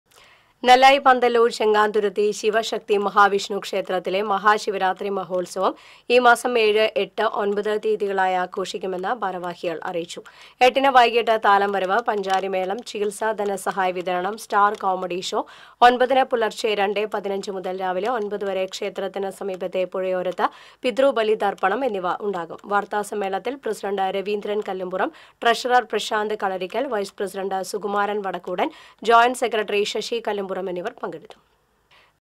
PANTHALLOOR ചെങ്ങാന്തുരുത്തി ക്ഷേത്രത്തില്‍ ശിവരാത്രി ആഘോഷം
നെല്ലായി പന്തല്ലൂര്‍ ചെങ്ങാന്തുരുത്തി ശിവശക്തി മഹാവിഷ്ണു ക്ഷേത്രത്തിലെ മഹാശിവ രാത്രി മഹോല്‍സവം ഈ മാസം ഏഴ്, എട്ട്, ഒമ്പത് തിയതികളിലായി ആഘോഷിക്കുമെന്ന് ഭാരവാഹികള്‍ വാര്‍ത്തസമ്മേളനത്തില്‍ അറിയിച്ചു. ഏഴിന് വൈകുന്നേരം ദേശത്തെ കലാപ്രതിഭകളുടെ കലാവിരുന്ന്, എട്ടിന് വൈകുന്നേരം മറ്റത്തൂര്‍ പള്ളമുറി യുവജനസംഘത്തിന്റെ നേതൃത്വത്തില്‍  ചെണ്ടുകാവടി, ശിങ്കാരിമേളം, തെയ്യാട്ടം എന്നിവയുടെ അകമ്പടിയോടെ താലംവരവ്, പഞ്ചാരിമേളം, ചികില്‍സ ധനസഹായ വിതരണം, ഉന്നതവിജയം നേടിയവരെ ആദരിക്കല്‍, സ്റ്റാര്‍കോമഡി ഷോ എന്നിവയുണ്ടാകും. ഒമ്പതിന് പുലര്‍ച്ചെ 2.15 മുതല്‍ രാവിലെ ഒമ്പതുവര ക്ഷേത്രത്തിനു സമീപത്തെ പുഴയോരത്ത് പിതൃബലിതര്‍പ്പണം നടക്കും. രാമചന്ദ്രന്‍ ഇയത്, ശിവന്‍ ശാന്തി എന്നിവര്‍ കാര്‍മികത്വം വഹിക്കും. വാര്‍ത്തസമ്മേളനത്തില്‍ പ്രസിഡന്റ് രവീന്ദ്രന്‍ കല്ലുംപുറം, ട്രഷറര്‍ പ്രശാന്ത് കളരിക്കല്‍, വൈസ് പ്രസിഡന്റ് സുകുമാരന്‍ വടക്കൂടന്‍, ജോയിന്റ്  സെക്രട്ടറി  ശശി കല്ലുംപുറം എന്നിവര്‍ പങ്കെടുത്തു .